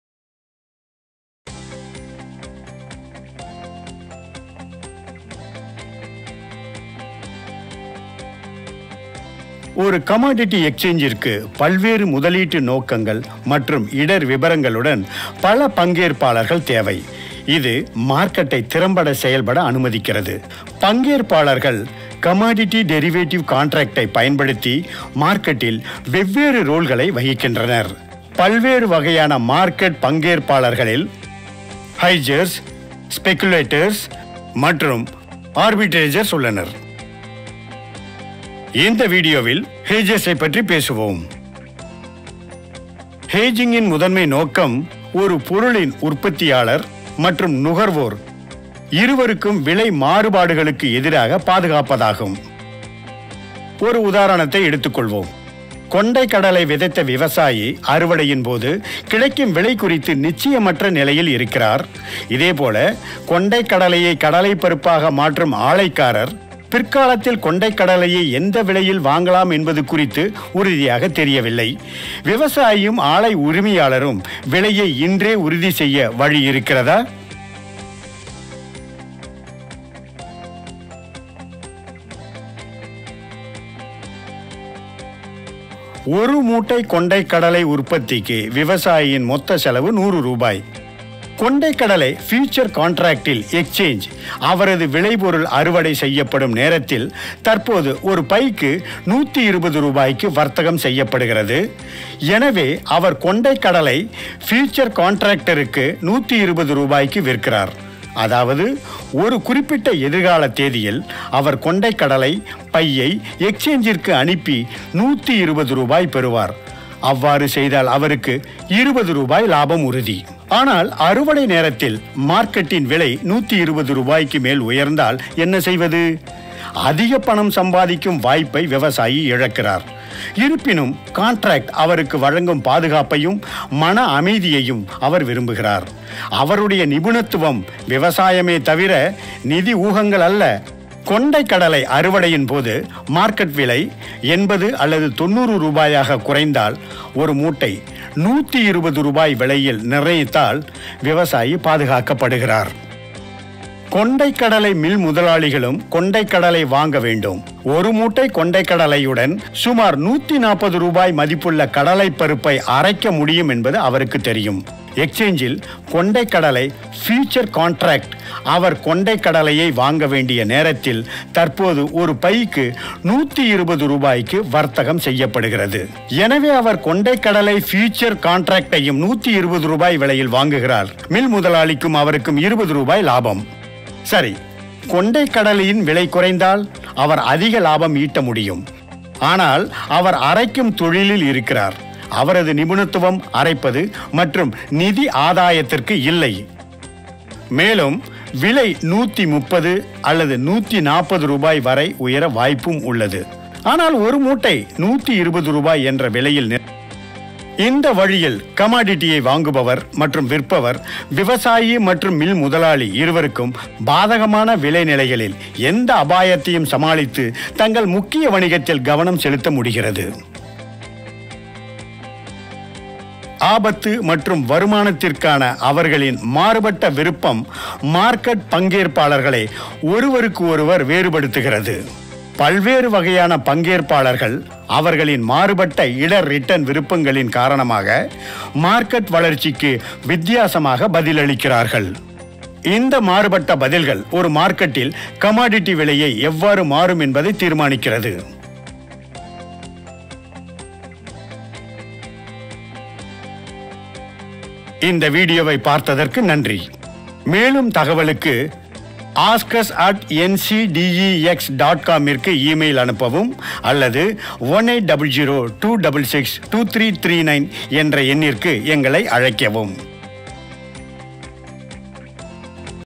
NCDEX IPFT concept video on Type of Market Participants - Hedgers (Tamil)
Learn the role of Hedgers in the commodity market ecosystem in this concept video.